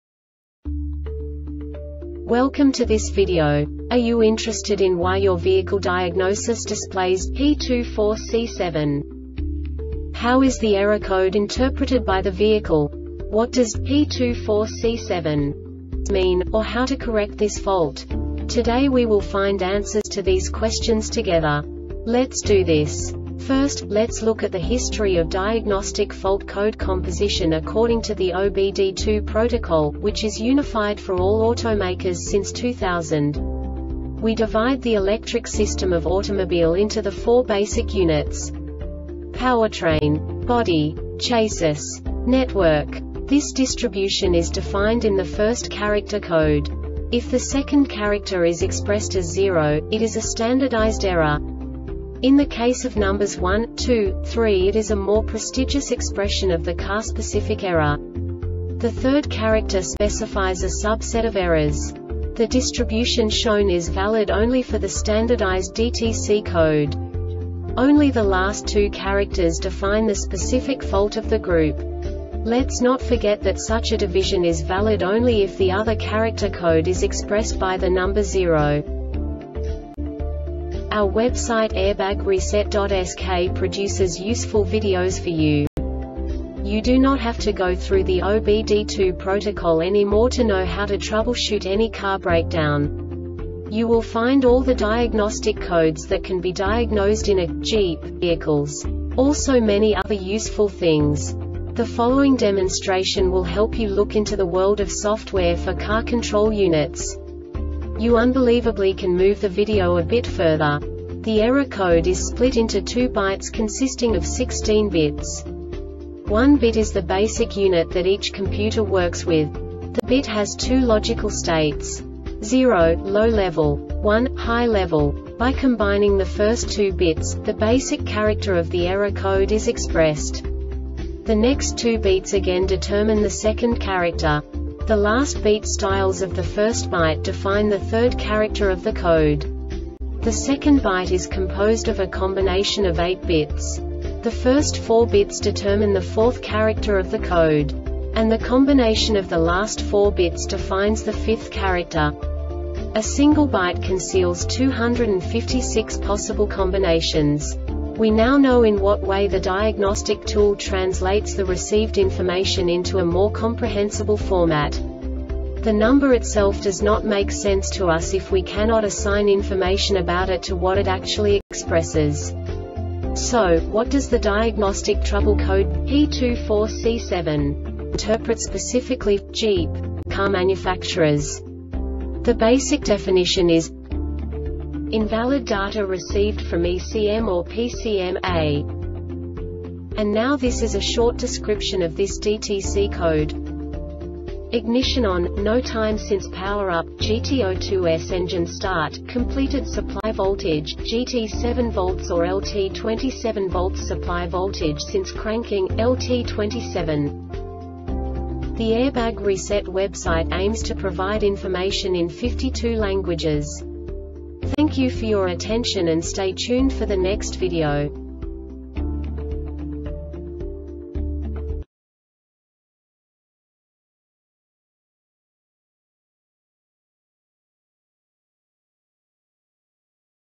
DTC Jeep P24C7 Short Explanation
Contents:
0:21 Basic DTC analysis according to OBD2 protocol standard.
1:48 Insight into programming
2:58 A diagnostically understandable description of the Diagnostic Trouble Code
3:05 Basic error definition

Make:
Jeep
Code:
P24C7
Definition:
Invalid Data Received From ECM or PCM "A"
Description:
Ignition on,noTime since power up,&gt;0.2 sEngine start,completedSupply voltage,&gt;7 V or 27 VSupply voltage since cranking,27 V